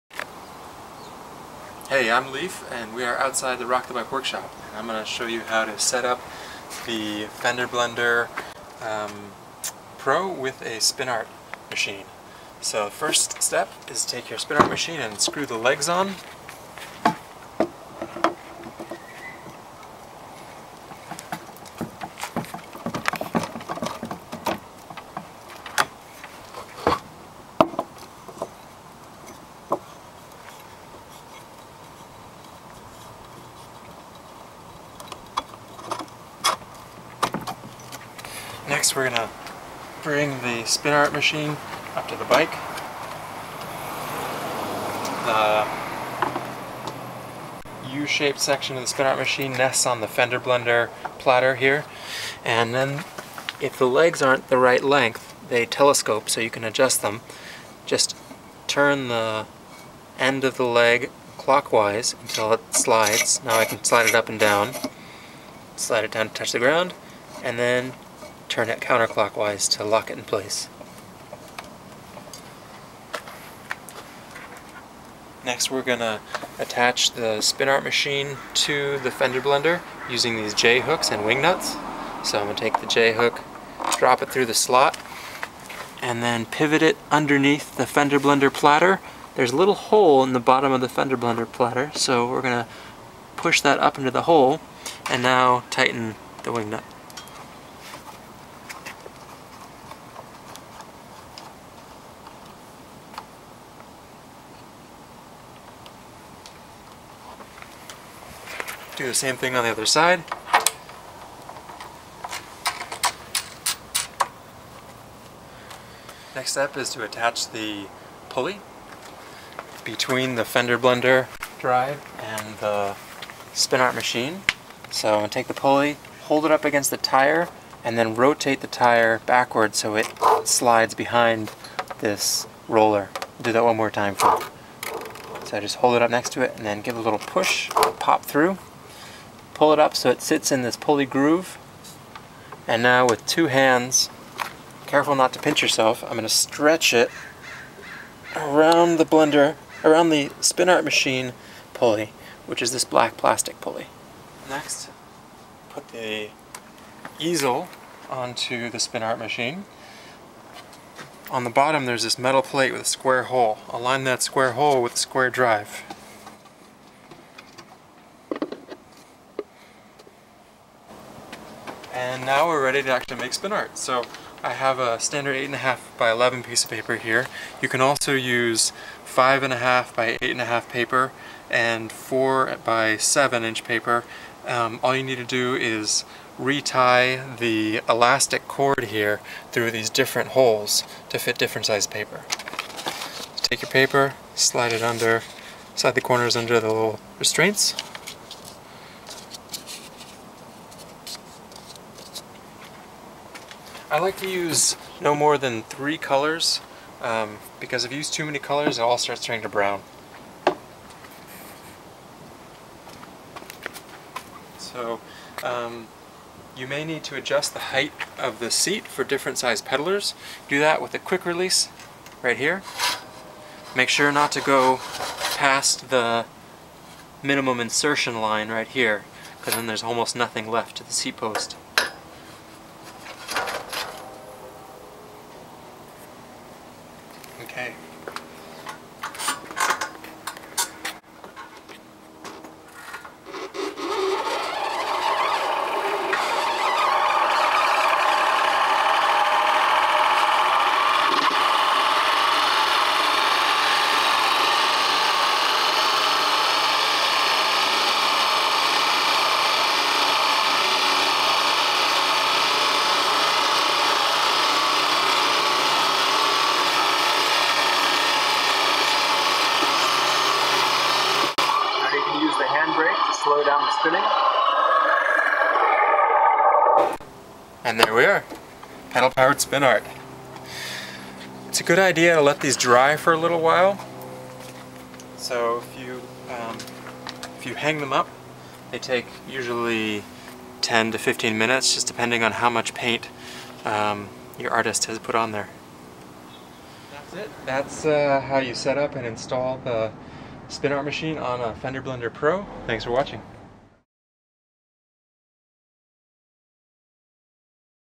Spin Art Machine setup & connecting to FBPro
Instructions for setup of the spin art machine from Rock The Bike.